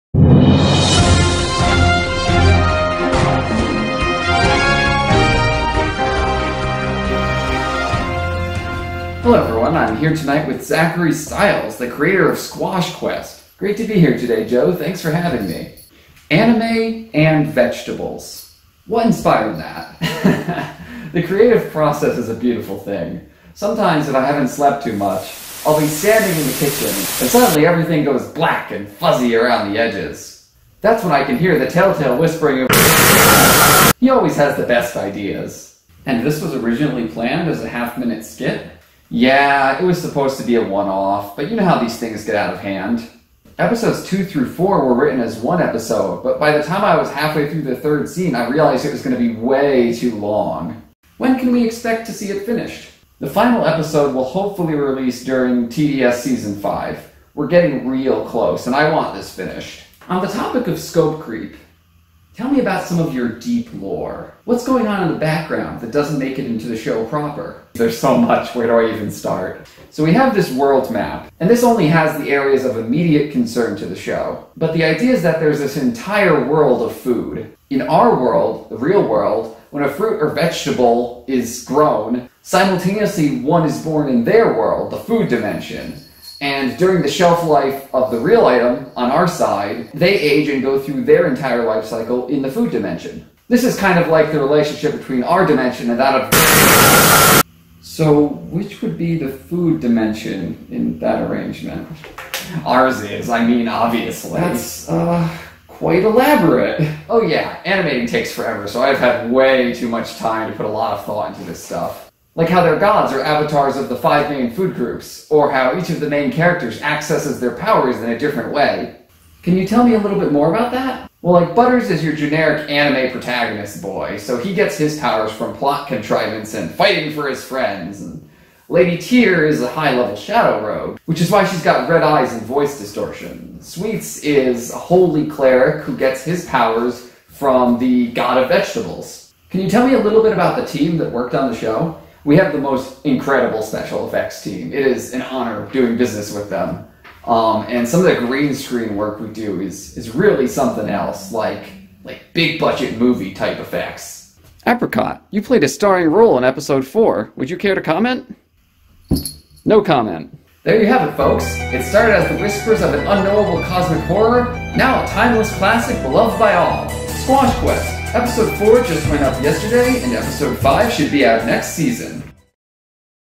The Making of Squash Quest
concerned onlooker, probably: "He's been playing action-figures with those vegetables for hours now.. do you think we should say something?"

me: "TODAY ON TDS News, we take you behind the scenes of the food-anime sensation that captured the hearts of a nation!"

Ironically I slept in and posted this nearly two hours late, so the time in the titlecard is wrong.